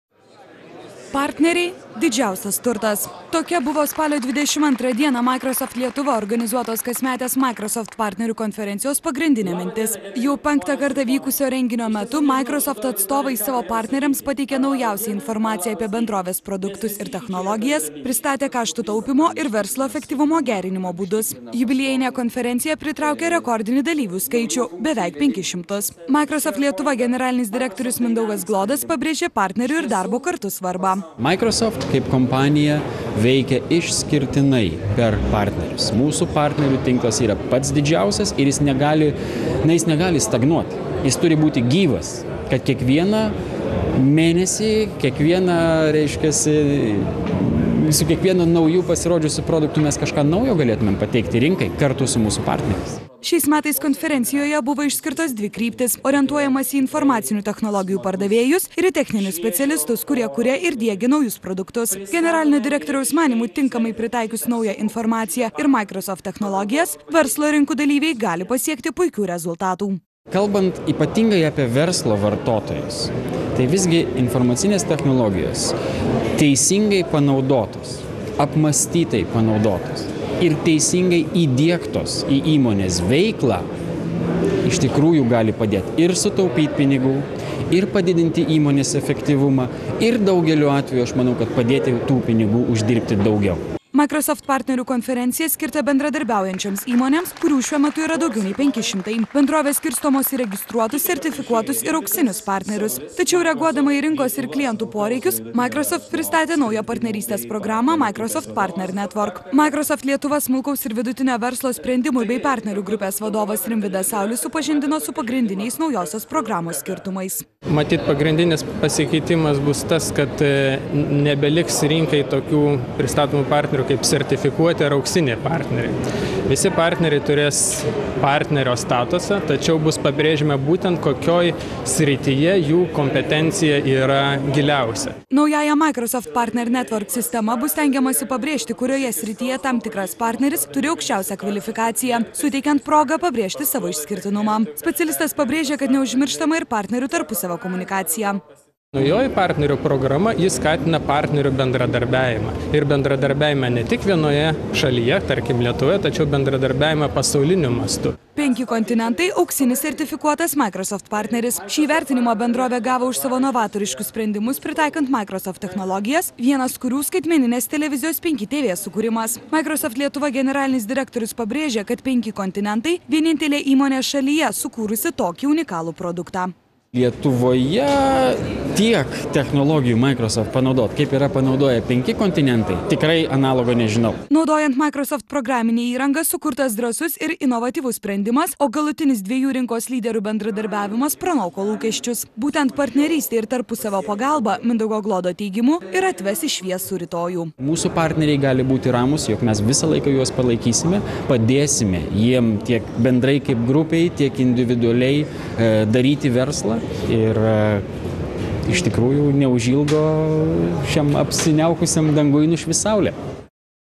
"Microsoft": partneriai didžiausias turtas
Partneriai  didžiausias turtas. Tokia buvo spalio 22 dieną "Microsoft Lietuva" organizuotos kasmetės "Microsoft" partnerių konferencijos pagrindinė mintis. Jau penktą kartą vykusio renginio metu "Microsoft" atstovai savo partneriams pateikė naujausią informaciją apie bendrovės produktus ir technologijas, pristatė kaštų taupymo ir verslo efektyvumo gerinimo būdus.